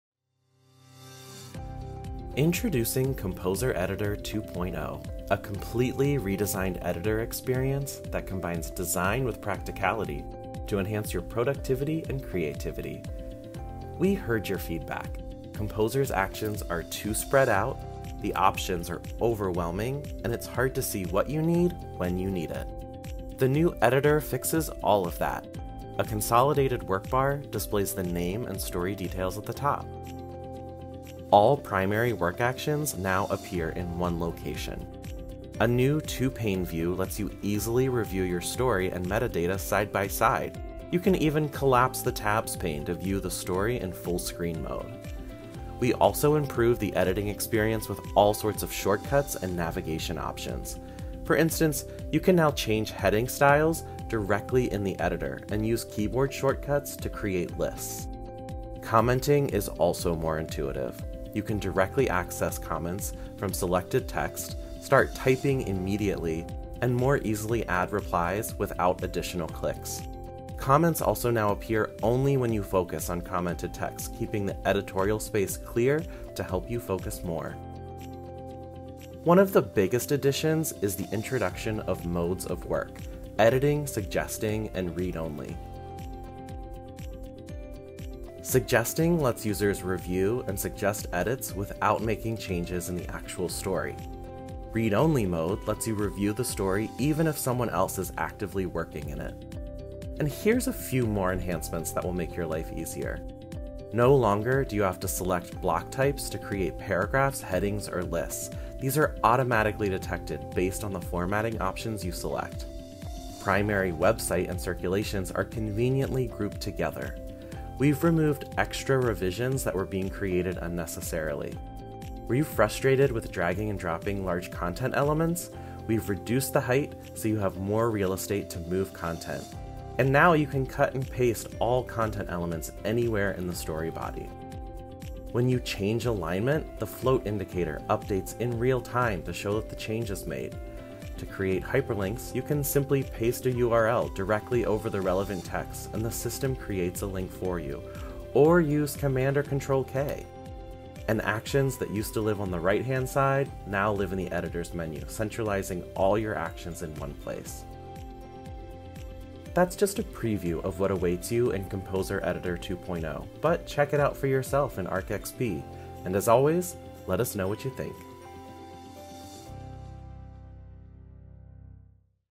Composer 2.0 Quick Demo | Redesigned editor experience to enhance productivity
Here's a quick demo of Arc XP's Composer 2.0.